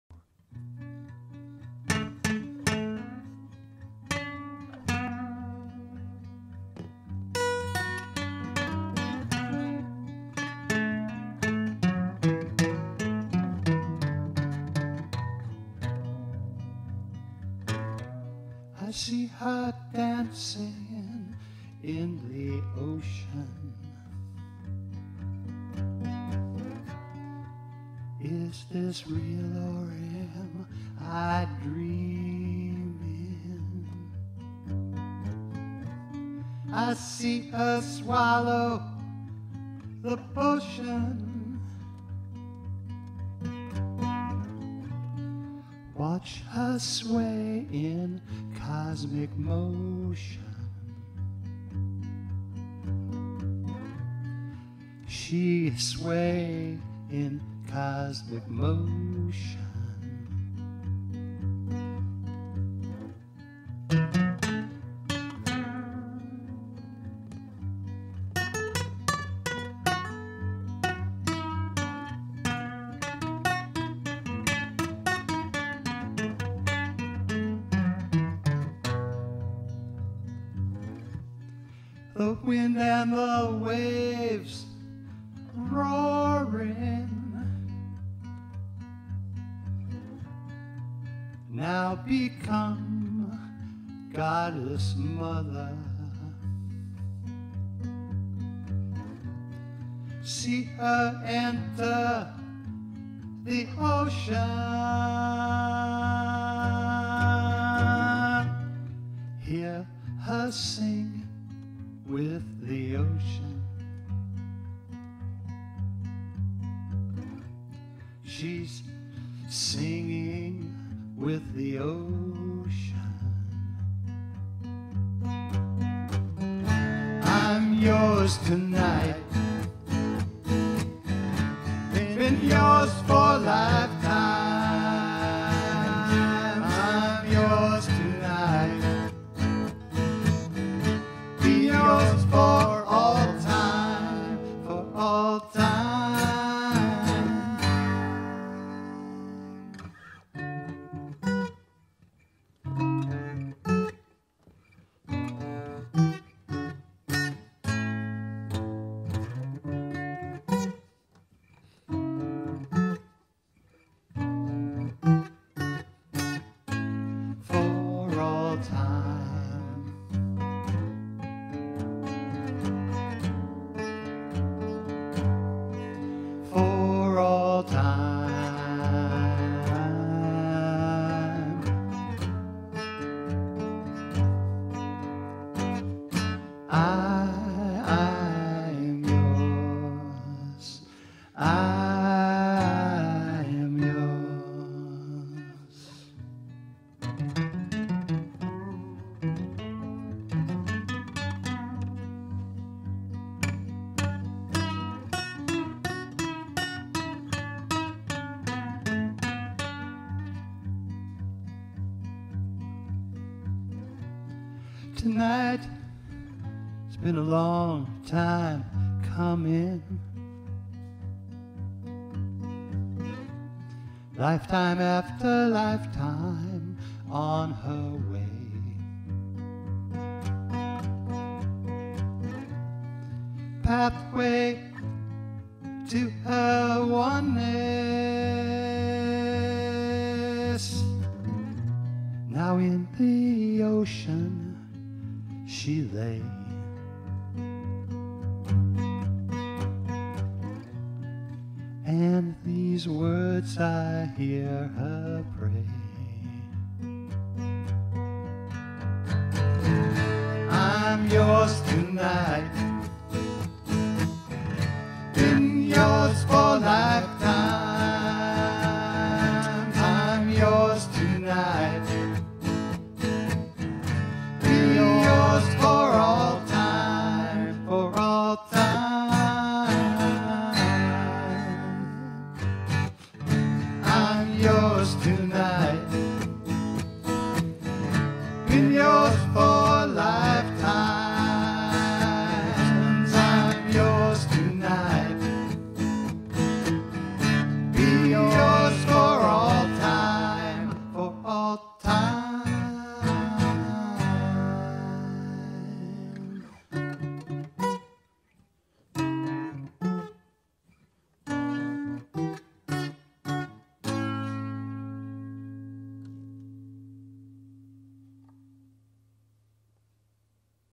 I'm Yours Tonight
It is a simple story of a woman who becomes one with the universe.